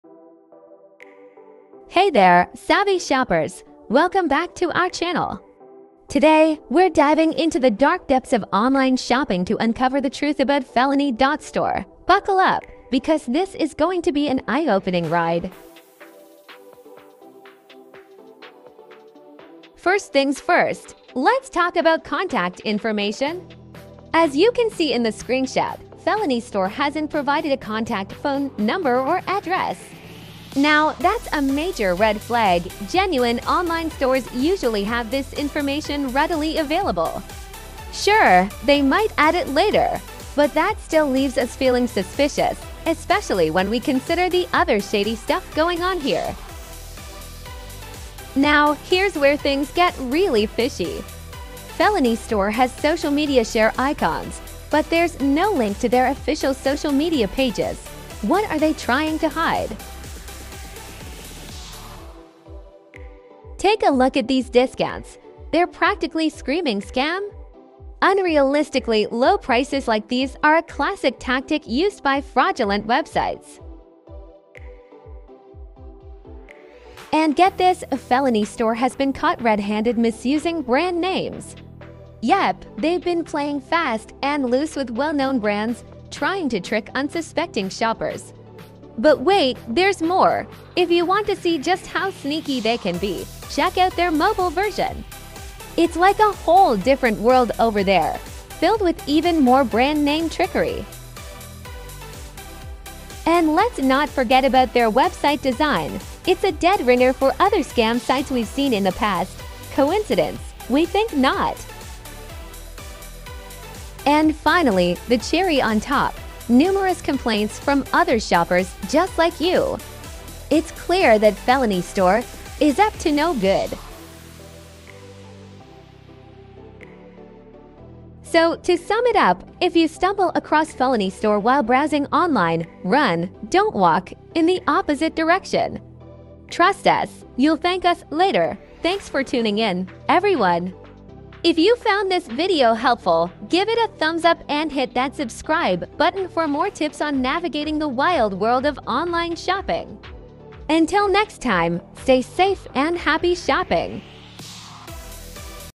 Unreal Discounts or Deceptive Scams? The Felony Store Investigation Revealed!
Is Felony.Store offering real discounts or deceptive scams? Our investigation reveals the shocking truth behind their deceptive practices and numerous customer complaints. Don't miss this eye-opening expose!